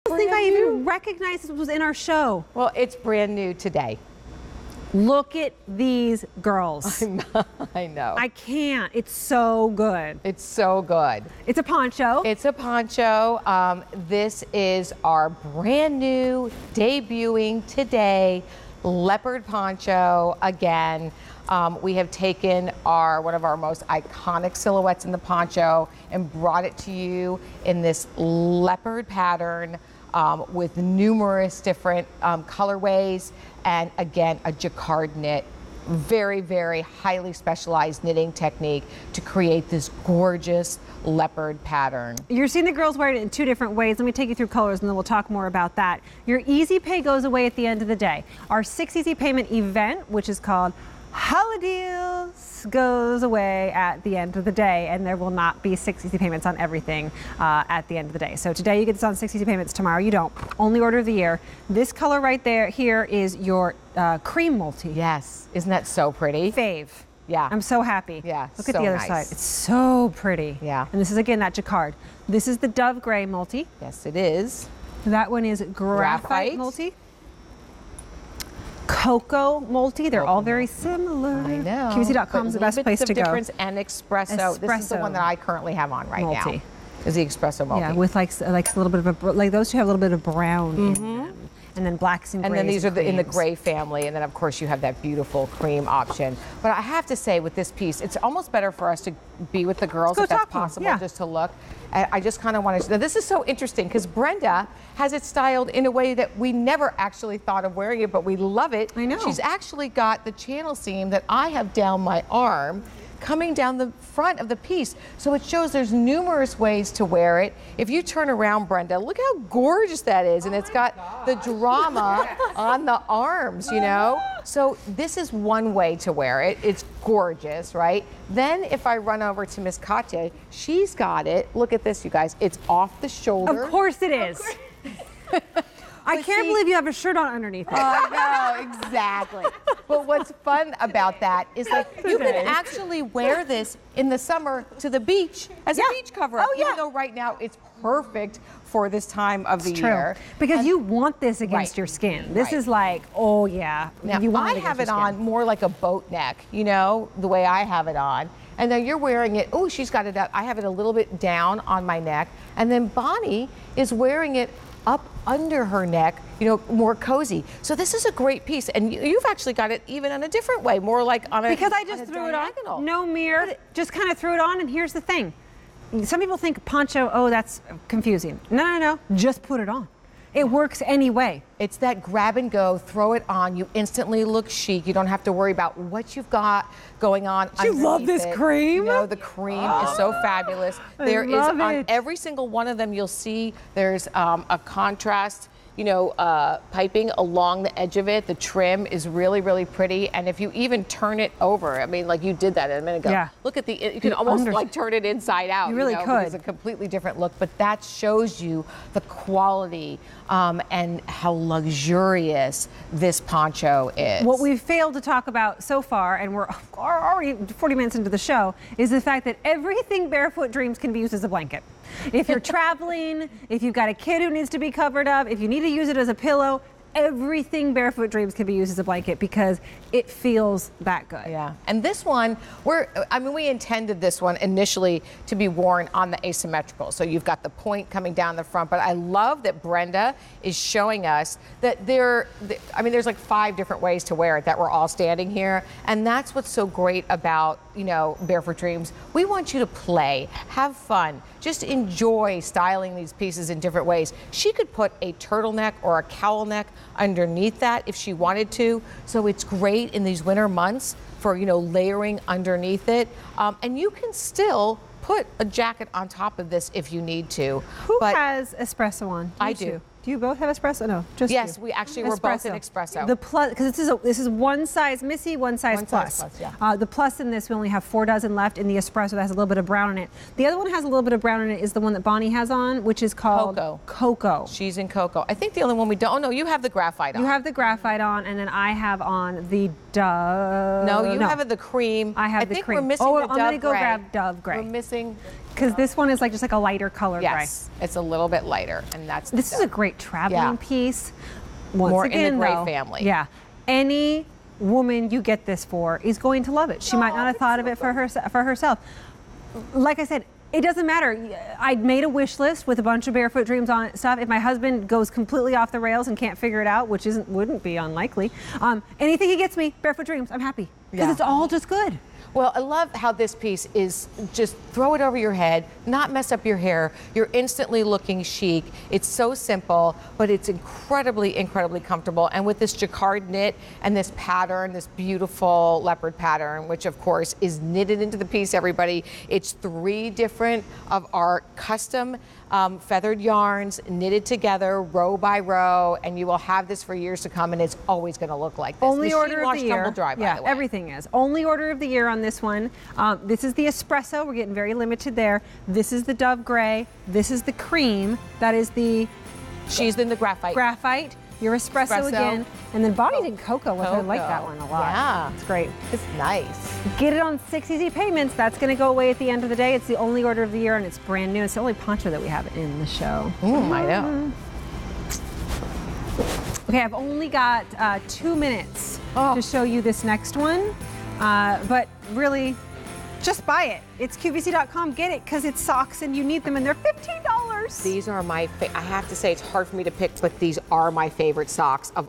Barefoot Dreams Cozychic Leopard Poncho on QVC
Barefoot Dreams Cozychic Leopard Poncho

The poncho -- a perfect cool-weather topper. Forgiving and flattering, this leopard-print poncho can be paired with anything from jeans to dress pants. From Barefoot Dreams.